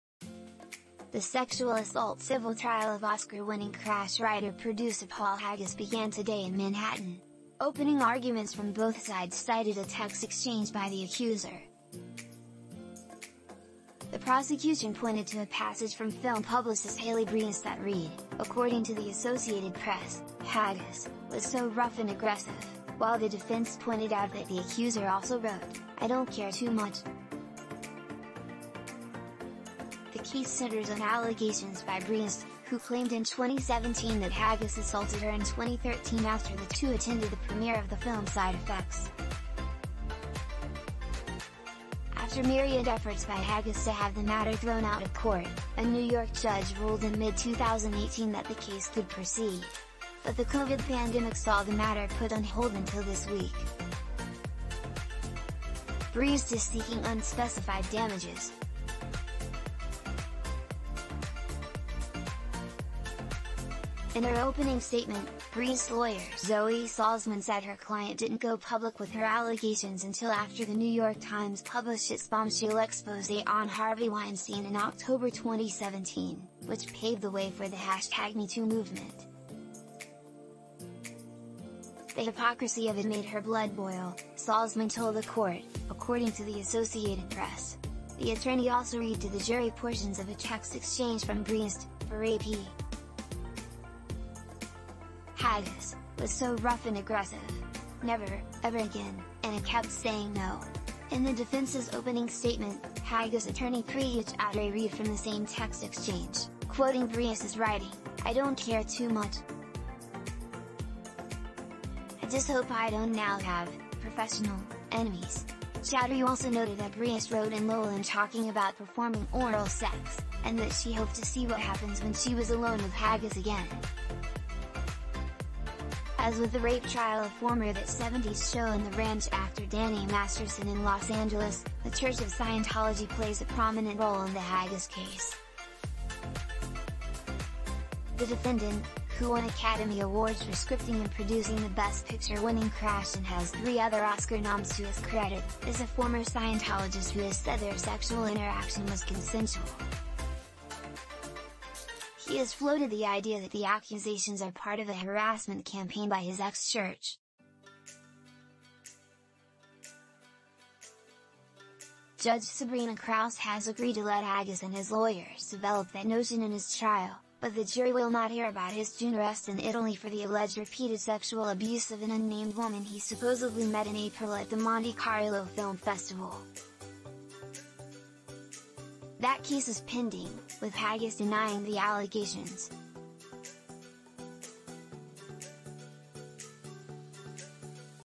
Paul Haggis Sexual Assault Civil Trial Opens With Both Sides Quoting
The sexual assault civil trial of Oscar-winning Crash writer-producer Paul Haggis began today in Manhattan. Opening arguments from both sides cited a text exchange by the accuser. The prosecution pointed to a passage from film publicist Haleigh Breest that read, according to the Associated Press, “[Haggis] was so rough and aggressive, ” while the defense pointed out that the accuser also wrote, “I don’t care too much. ”The case centers on allegations by Breest, who claimed in 2017 that Haggis assaulted her in 2013 after the two attended the premiere of the film Side Effects. After myriad efforts by Haggis to have the matter thrown out of court, a New York judge ruled in mid-2018 that the case could proceed. But the Covid pandemic saw the matter put on hold until this week. Breest is seeking unspecified damages. In her opening statement, Breest’s lawyer Zoe Salzman said her client didn’t go public with her allegations until after The New York Times published its bombshell exposé on Harvey Weinstein in October 2017, which paved the way for the MeToo movement. “The hypocrisy of it made her blood boil, ” Salzman told the court, according to the Associated Press. The attorney also read to the jury portions of a text exchange from Breest, per AP. “[Haggis] was so rough and aggressive. Never, ever again … And I kept saying no. ”In the defense’s opening statement, Haggis attorney Priya Chaudhry read from the same text exchange, quoting Breest as writing, “I don’t care too much. I just hope I don’t now have [professional] enemies. ” Chaudhry also noted that Breest wrote an “lol” in talking about performing oral sex, and that she hoped to “see what happens” when she was alone with Haggis again. As with the rape trial of former That ’70s Show and The Ranch actor Danny Masterson in Los Angeles, the Church of Scientology plays a prominent role in the Haggis case. The defendant, who won Academy Awards for scripting and producing the Best Picture-winning Crash and has three other Oscar noms to his credit, is a former Scientologist who has said their sexual interaction was consensual. He has floated the idea that the accusations are part of a harassment campaign by his ex-church. Judge Sabrina Krause has agreed to let Haggis and his lawyers develop that notion in his trial, but the jury will not hear about his June arrest in Italy for the alleged repeated sexual abuse of an unnamed woman he supposedly met in April at the Monte Carlo Film Festival. That case is pending, with Haggis denying the allegations.